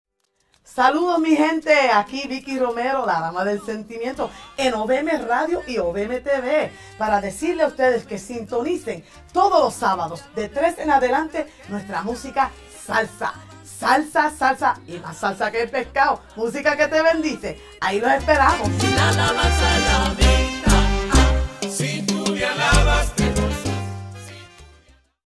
Salsa Cristiana, todos los sábados aquí en OVMRadio desde las 3 pm
🎶 Para animar tu fin de semana! Todos los sábados, disfruta de la mejor Salsa Cristiana, aquí en OVMRadio desde las 3 pm hasta las 4:30 pm.